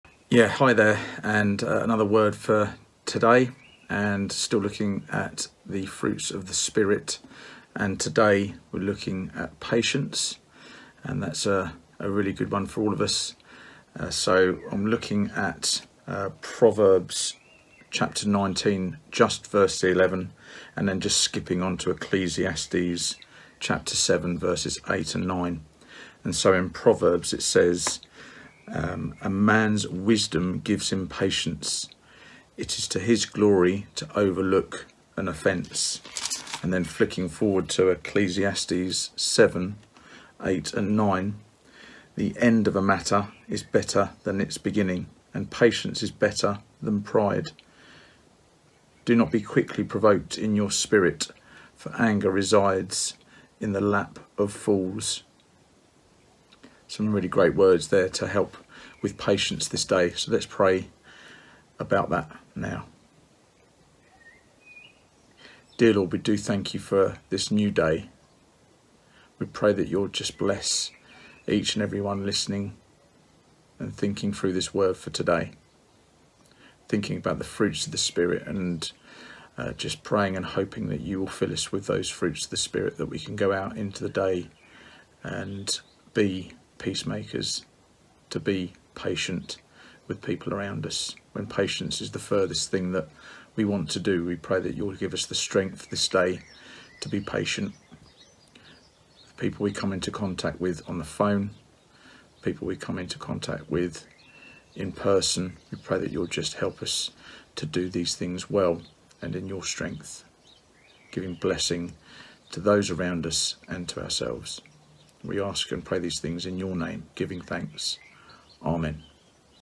Word for Today 10/6
Today, Rob Rouse reads from Proverbs 19:11 & Ecclesiastes 7:8-9